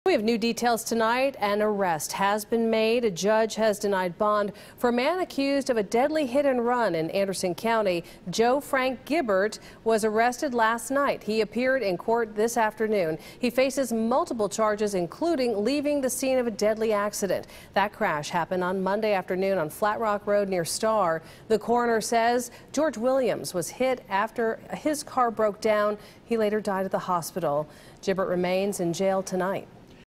Arrest made in deadly hit-and-run
A man has been charged in a fatal hit and run accident that killed an Iva man.